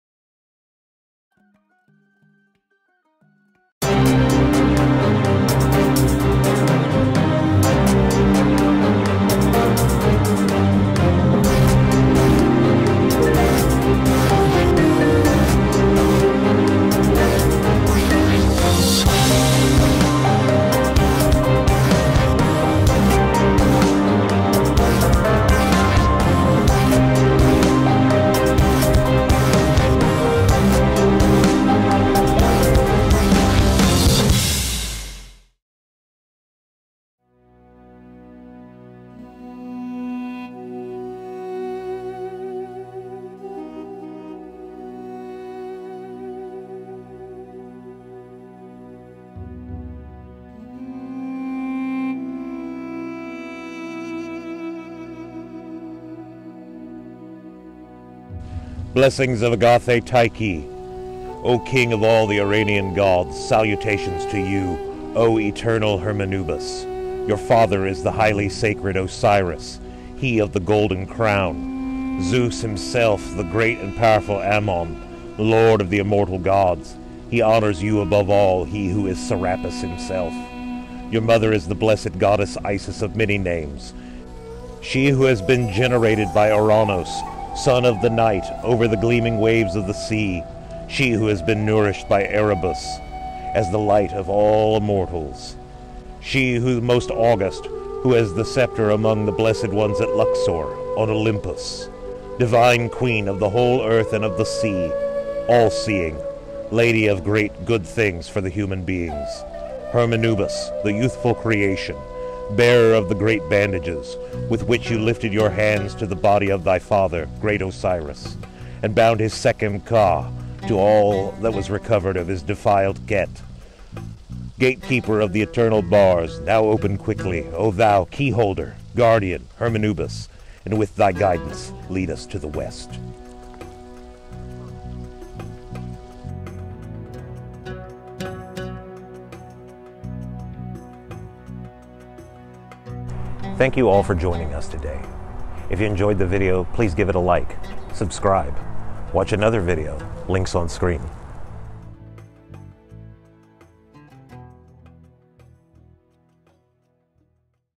A Pagan Prayer to Hermanubis
Intonation of an Antique Egyptian Hellenistic Kemetic Pagan prayer to the Ptolemaic syncretic god, Hermanubis.
Prayer is constructed from a First Century inscription from Kios in Bithynia, combined with excepts from "The Greek Magical Papyri" as translated by Hans Dieter Betz.
Performed at the the Igleheart Mausoleum in Evansville, Indiana's Oak Hill Cemetery by D℞ Wilke.